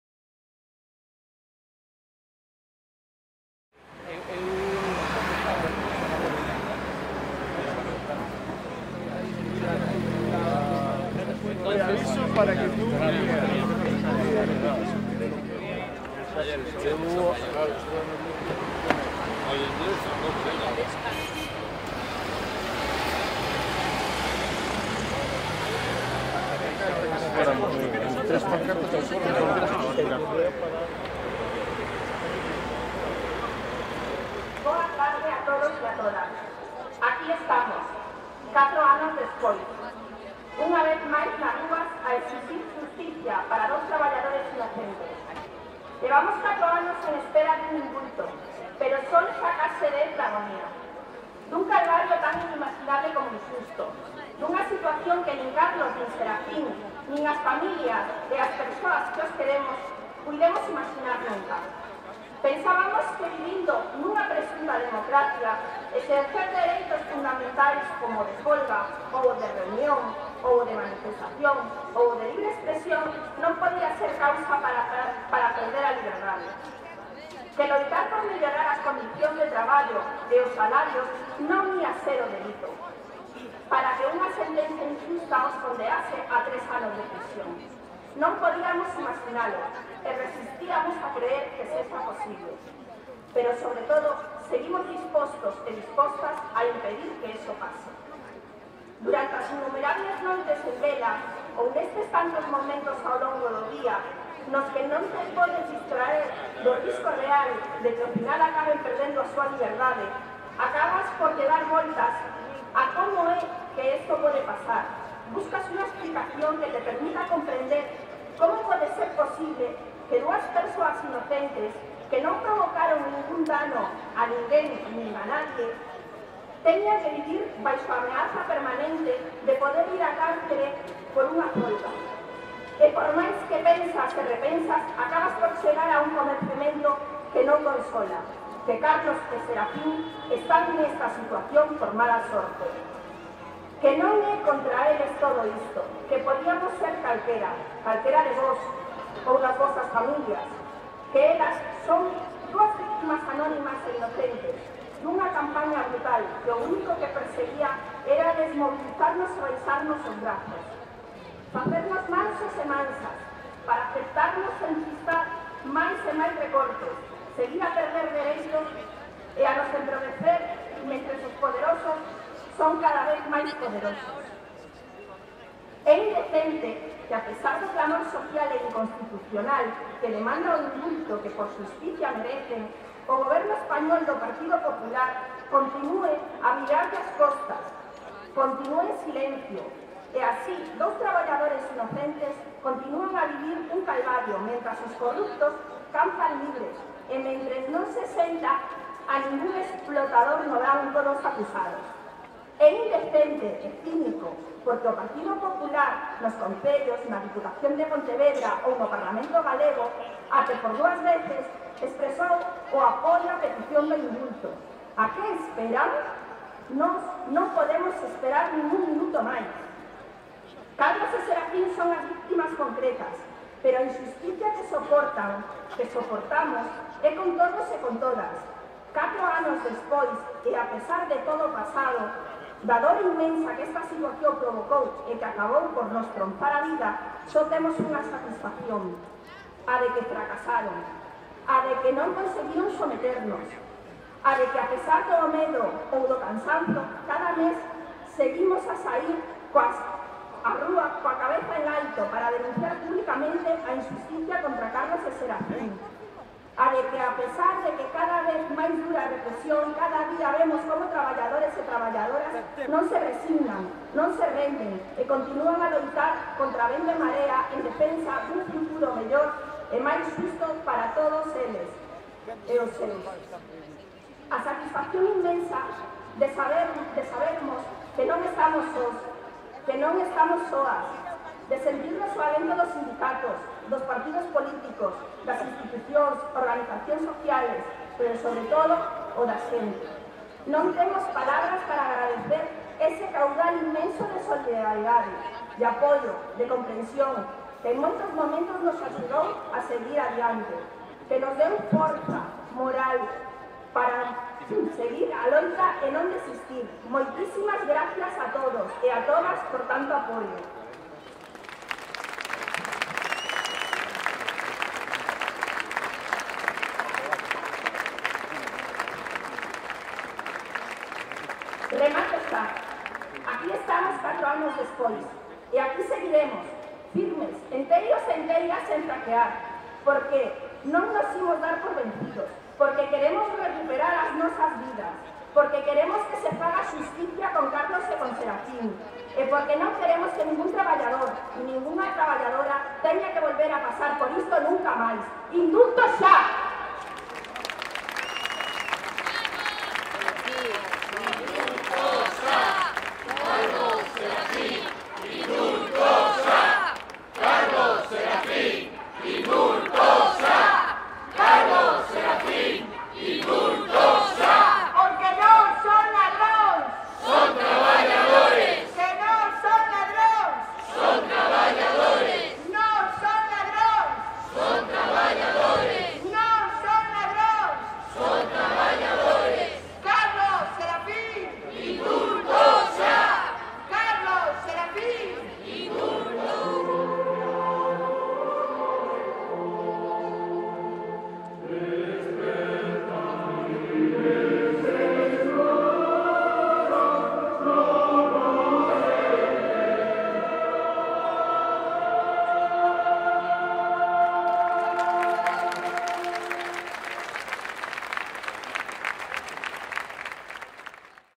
Manifestación indulto Carlos y Serafin en Vigo 22/02/2018
La gente lleva ya cuatro años saliendo a la calle para reclamar la medida de gracia, pero, desgraciadamente, la gente tendrá que seguir saliendo a la calle, porque la medida de gracia no llega.
4 años pidiendo el indulto, por algo que dicen que ocurrió hace casi una década.